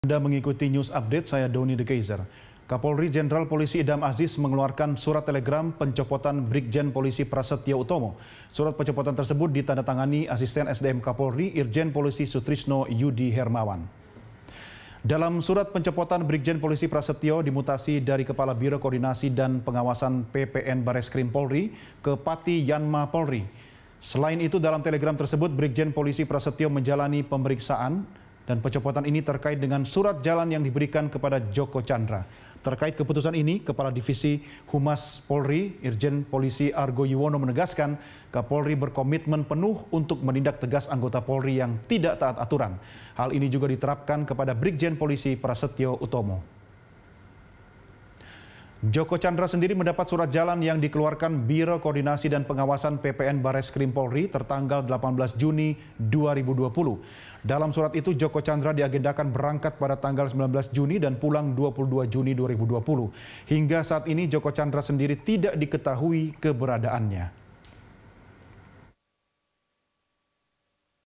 Kapolri Copot Jenderal Pembuat Surat Jalan Djoko Tjandra
Kapolri Jenderal Idham Azis mengeluarkan surat telegram pencopotan Brigjen Prasetijo Utomo. Surat pencopotan tersebut ditandatangani AS-SDM Kapolri Irjen Sutrisno Yudi Hermawan.